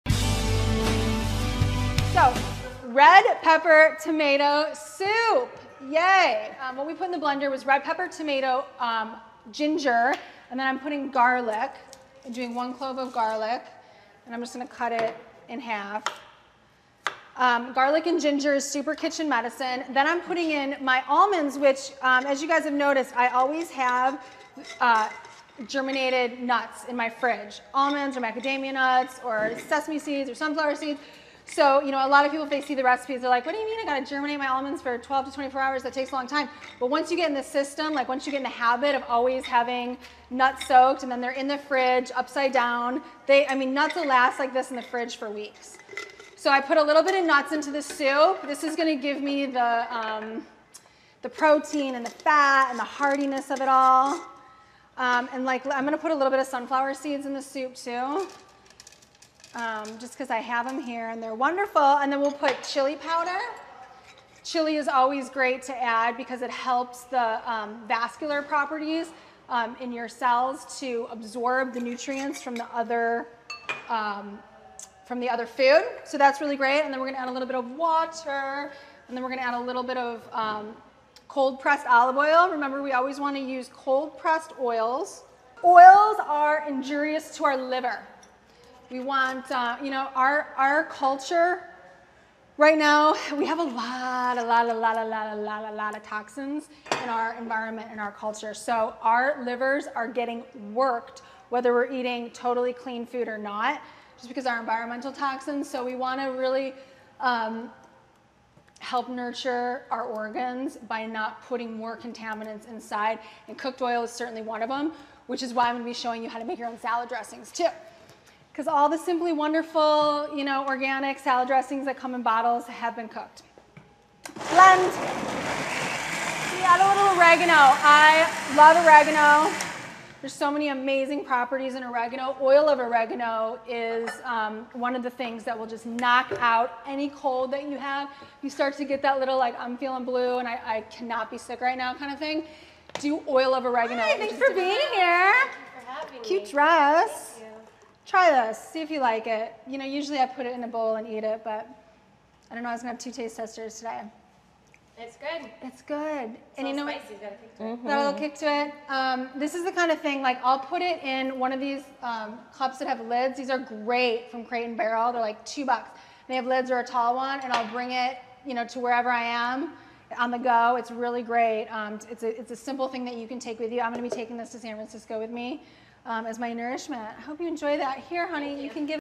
Raw Food Recipe Demo- Raw Tomato & Red Pepper Soup- "Blythe Raw Live"
Blythe Metz show you how to make a delicious raw tomato and red pepper soup.